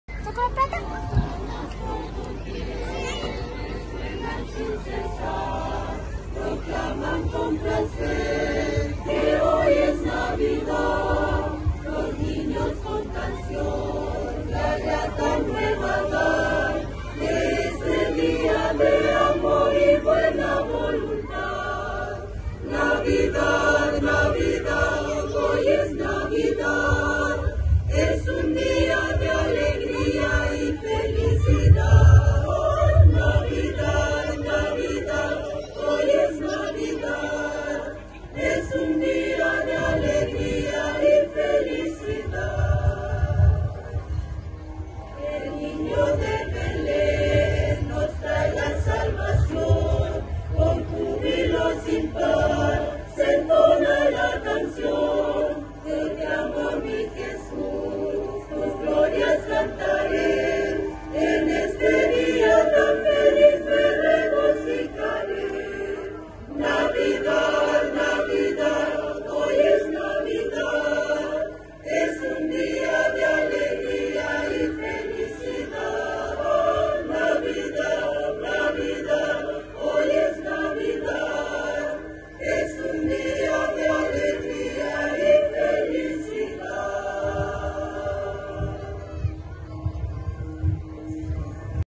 Cantata Navideña coro Iglesia Evangélica Pentecostal Carahue, Chile
Cantata Navideña coro Iglesia Evangélica Pentecostal Carahue, Chile. Sábado 23 de Diciembre de 2023